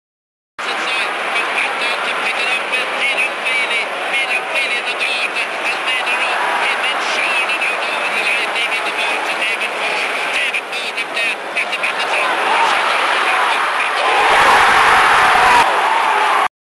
A Goal for Clare!
Clare v Waterford, All-Ireland Hurling Semi-Final, 2002.